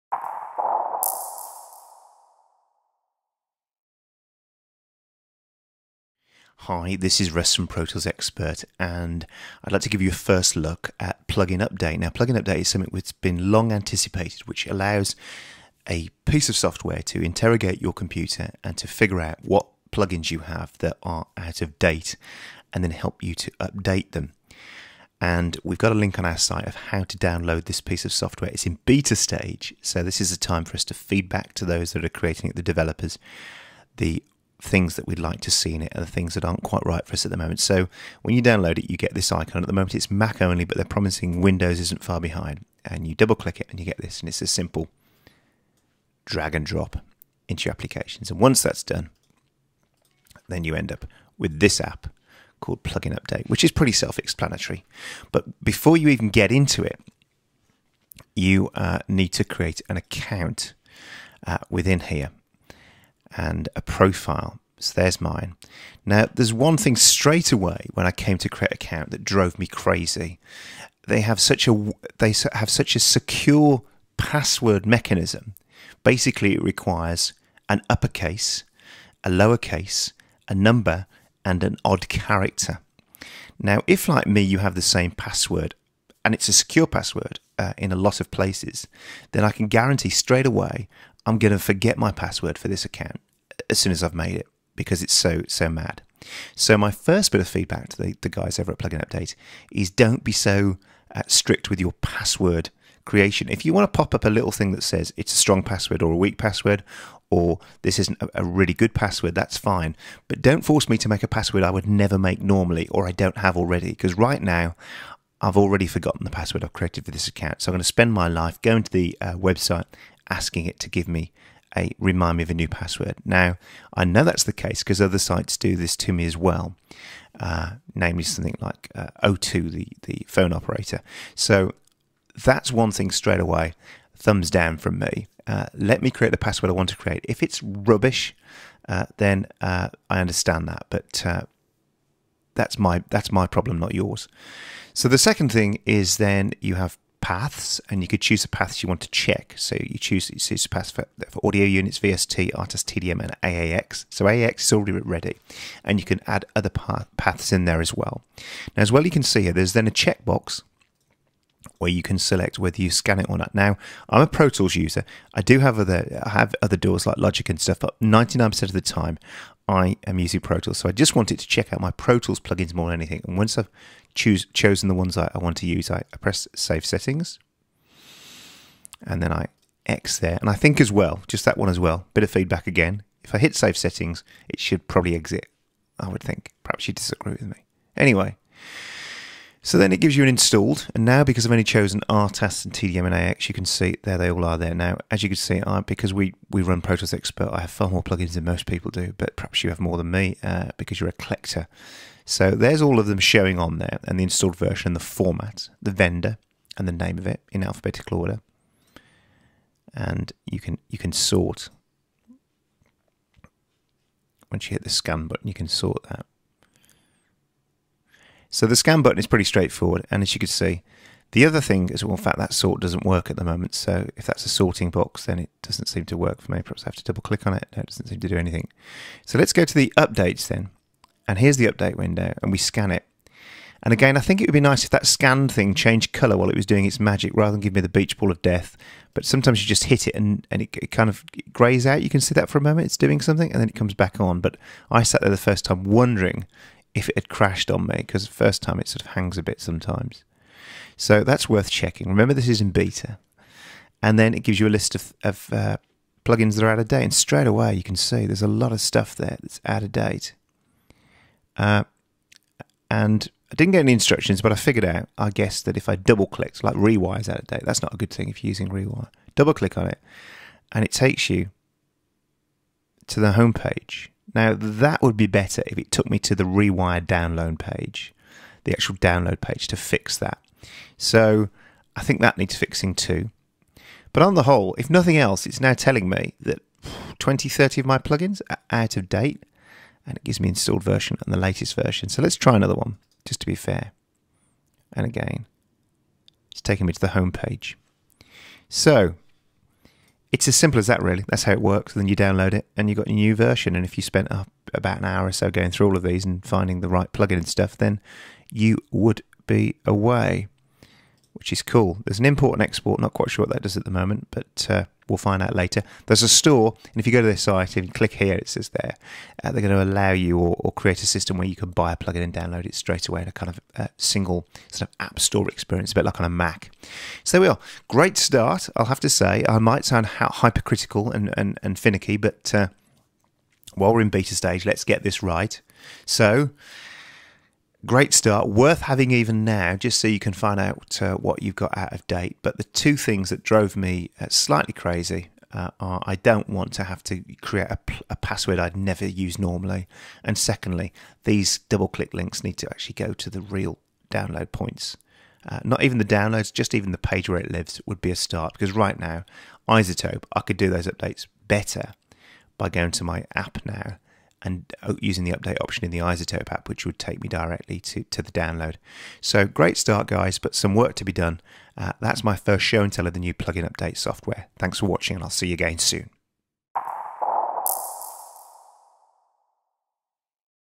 Show & Tell Of New PluginUpdate App Beta - Video Review
Russ takes a look at the public beta of the new PluginUpdate App Beta from Kazrog.